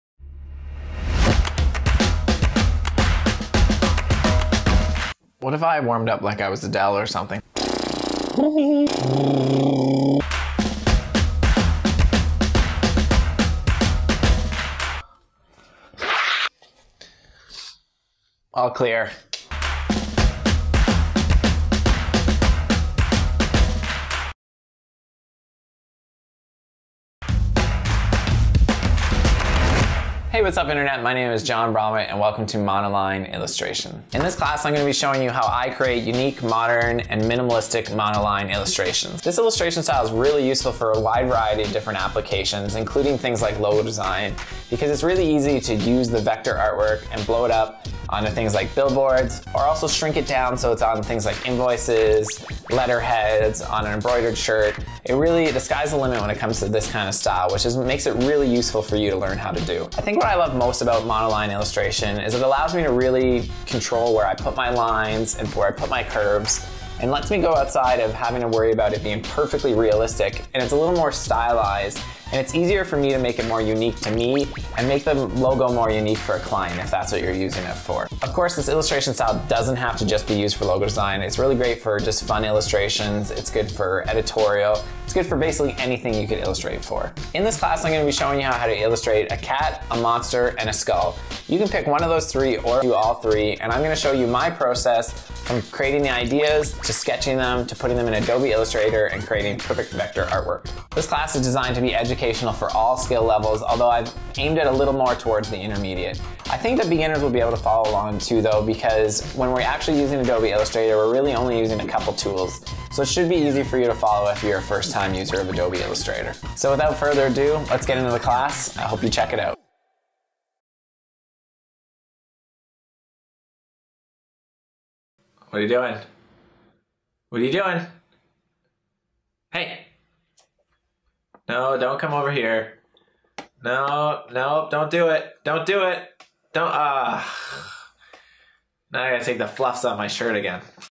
Skillshare Class Trailer: Monoline Illustration In Adobe Illustrator
Monoline Illustration is a great technique for creating really modern, minimalistic illustrations that can be used for a wide variety of applications. Because of their vector nature, they can easily be used for logos, branding, editorial, web, icons, and just about any purpose you might have for an illustration. In this class I will be showing you my process from references images, to finding inspiration, sketching, and finally, vectorizing the art in Adobe Illustrator. This class is aimed for Intermediate users of Adobe Illustrator, but even beginners may be able to follow along as I do not use many tools in the software. However, there is a lot to learn in the idea and sketch phase as well!I hope you will check out the class and let me know what you think! Make sure to go to my profile and follow me to see more.